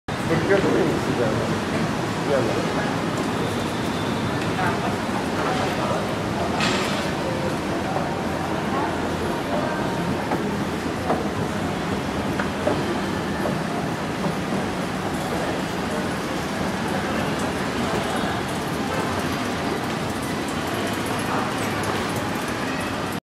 Plaza Festival, Jakarta: Small Brand New Pillar Escalator
These Pillar Escalator at Plaza Festival Jakarta. These Escalator are outside of the lobby.